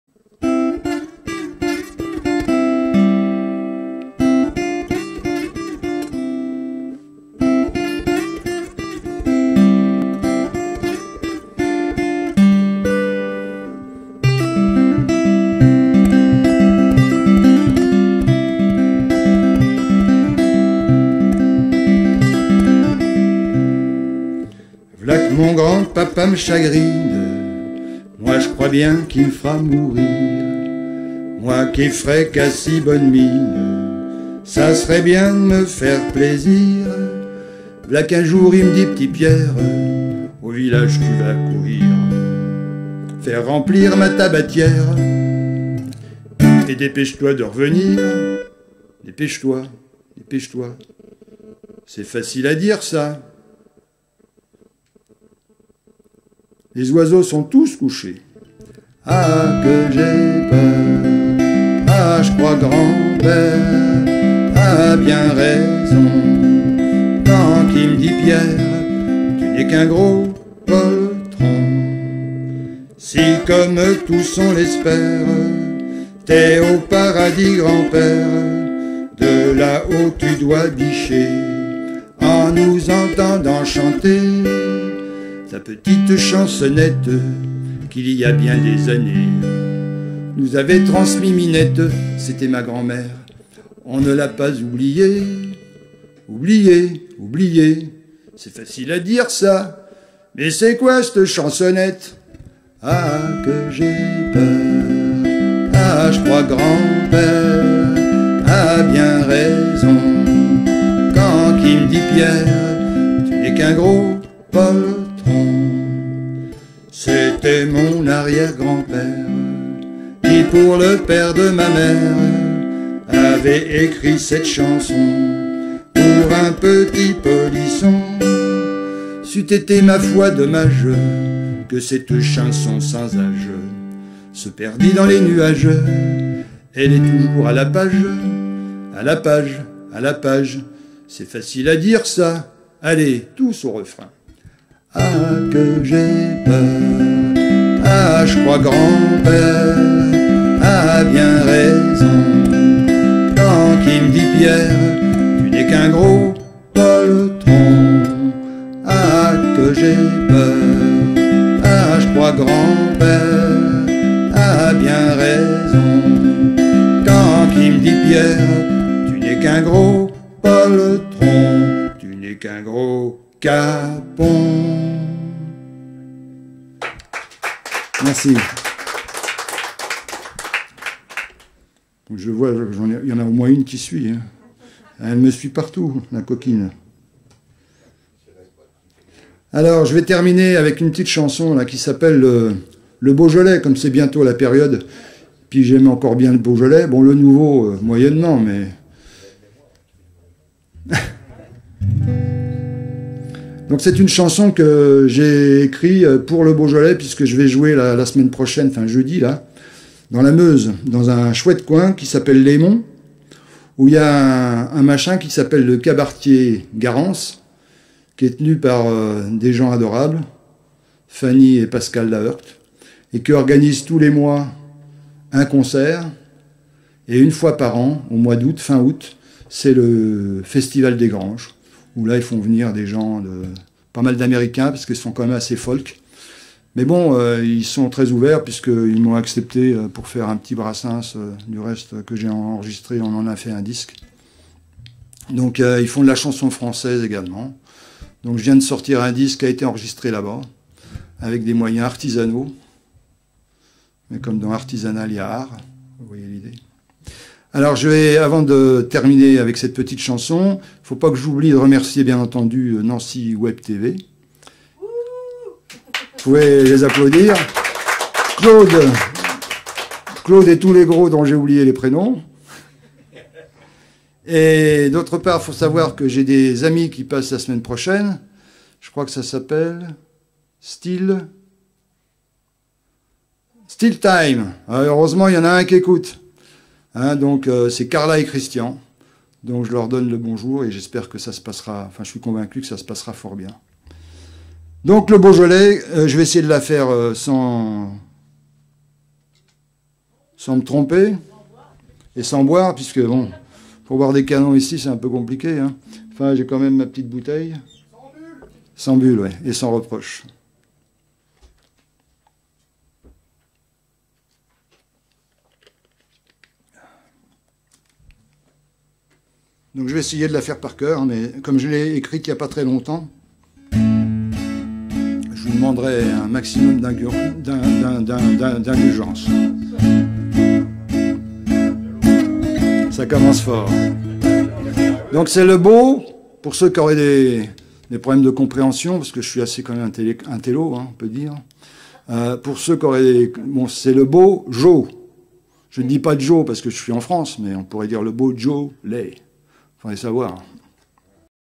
12 Pour mon arrière grand père
Captation de l'émission en direct consacrée à Martial ROBILLARD diffusée dimanche 16 novembre 2014 depuis le plateau de